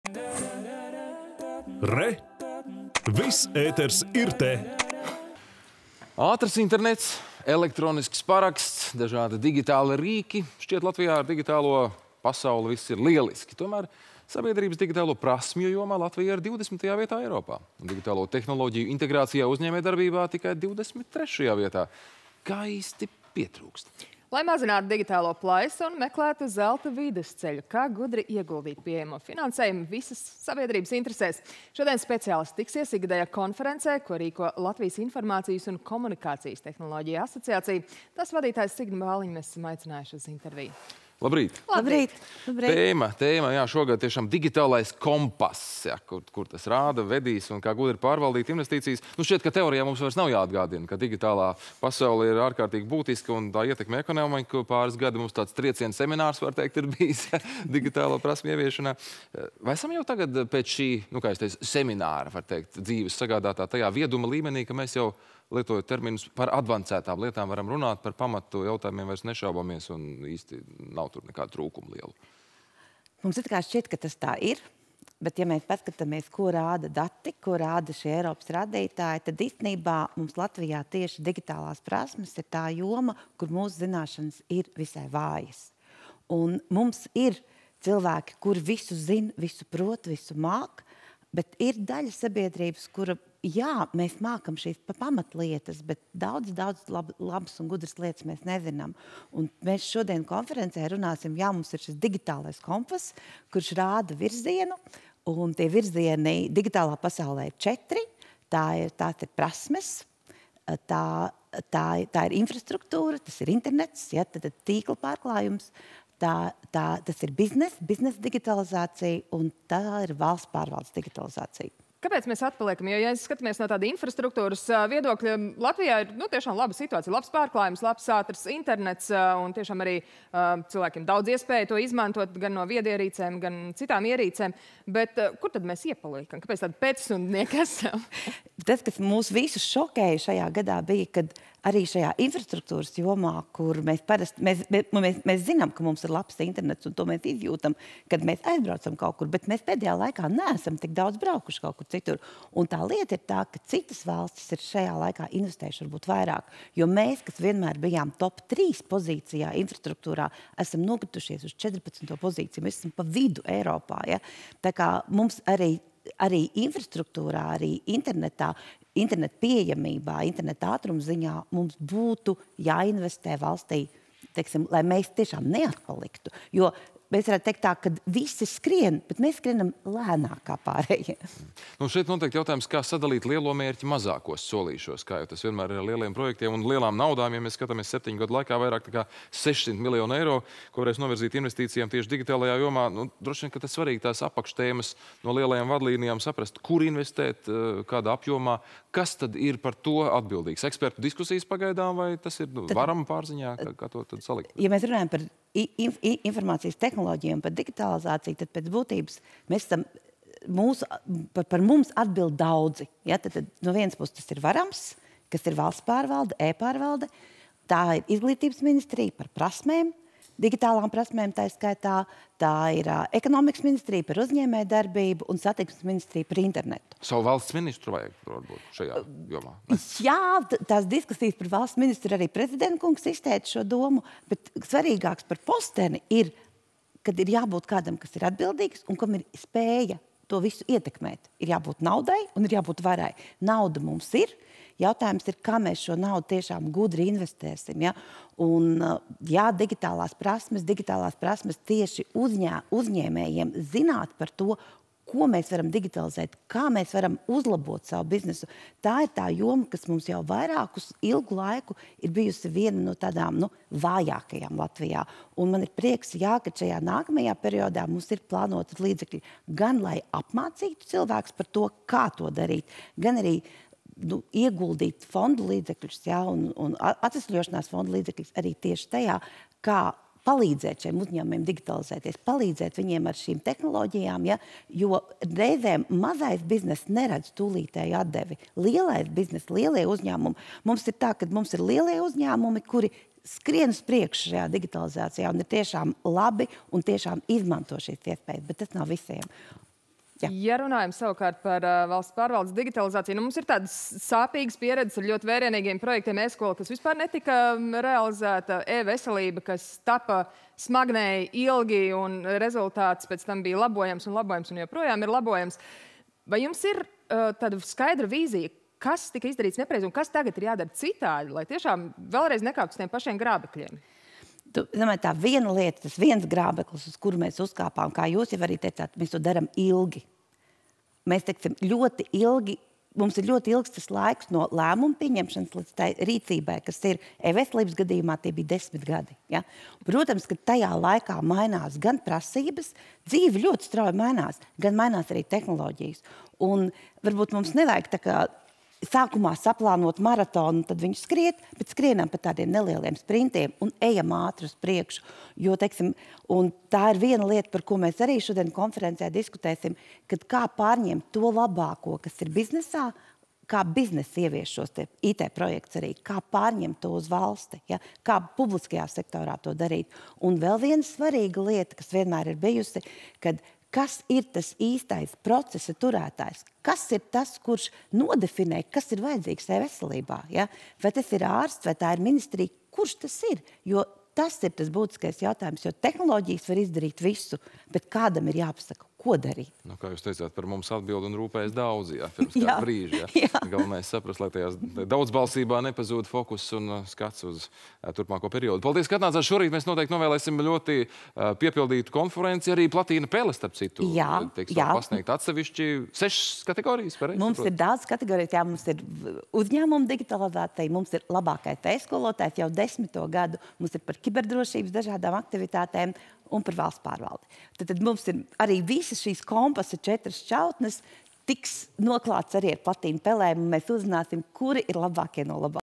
Intervija ar LIKTA prezidenti Signi Bāliņu
Ātrs internets; elektronisks paraksts; dažādi digitāli rīki. Šķiet, Latvijā ar digitālo pasauli viss ir lieliski, tomēr - sabiedrības digitālo prasmju jomā Latvija ir 20. vietā Eiropā; digitālo tehnoloģiju integrācijā uzņēmējdarbībā – tikai 23.vietā… Kā īsti pietrūkst?
Lai mazinātu „digitālo plaisu” un meklētu zelta vidusceļu, kā gudri ieguldīt pieejamo finansējumu visas sabiedrības interesēs, šodien speciālisti tiksies ikgadējā konferencē, ko rīko Latvijas informācijas un komunikācijas tehnoloģijas asociācija. Tās vadītāju Signi Bāliņu esam aicinājuši uz interviju.